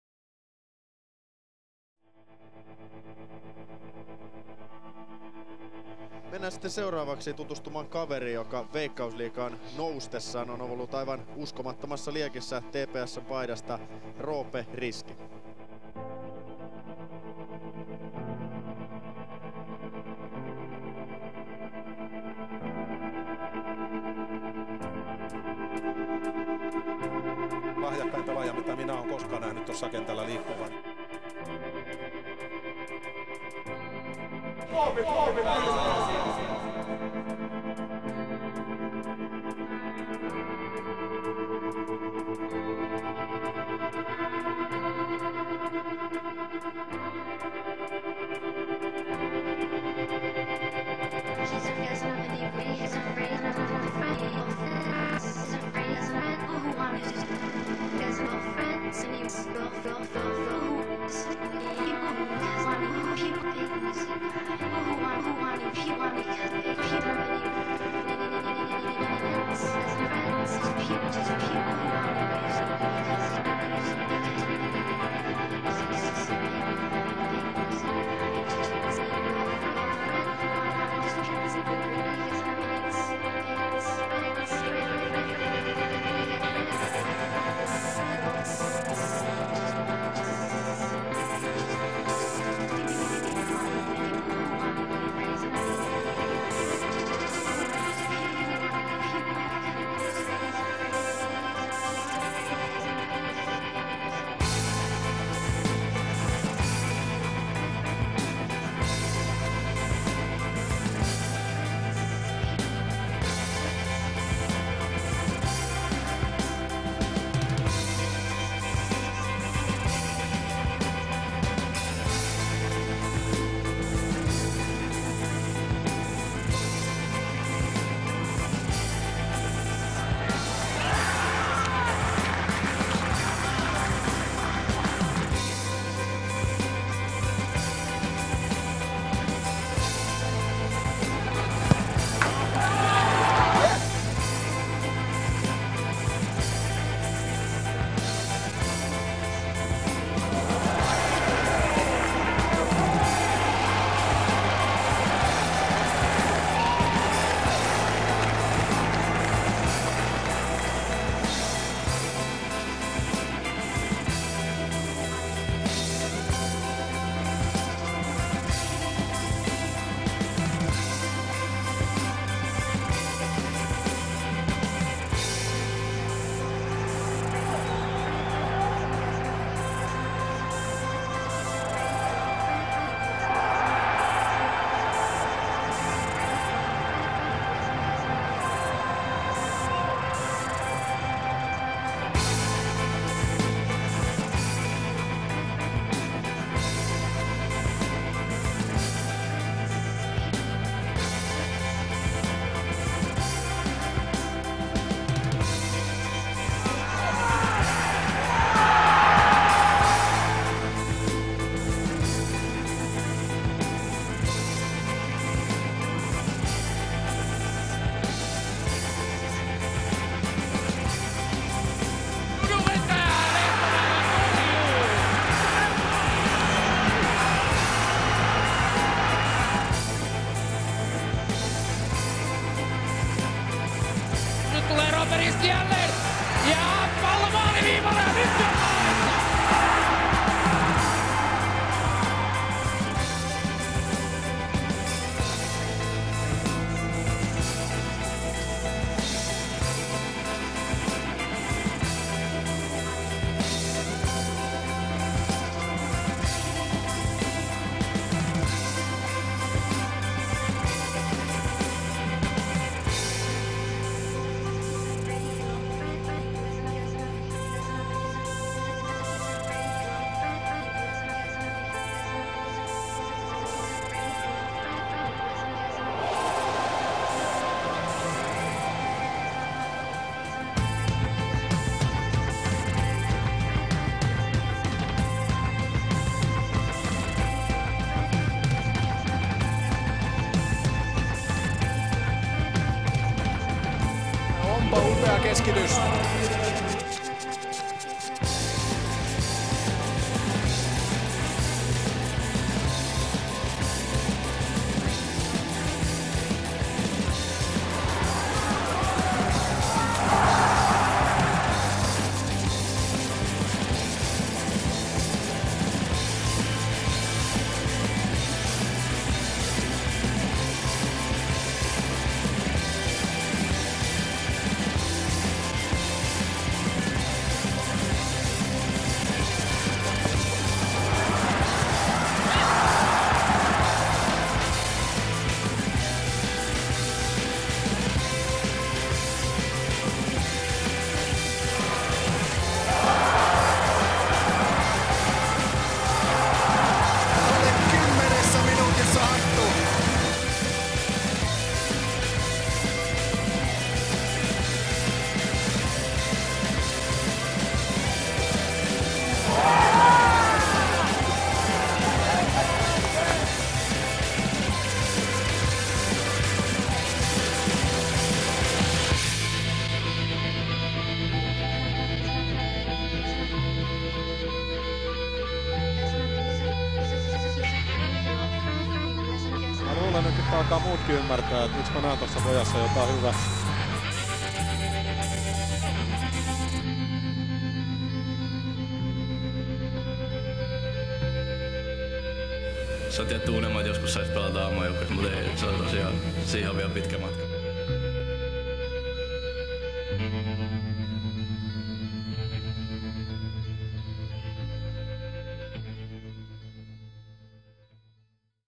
Roope Riski #17 - TPS 2010
Huippuhetket Roopen ensimmäisestä kaudesta Veikkausliigassa. Videon teki Tumpero.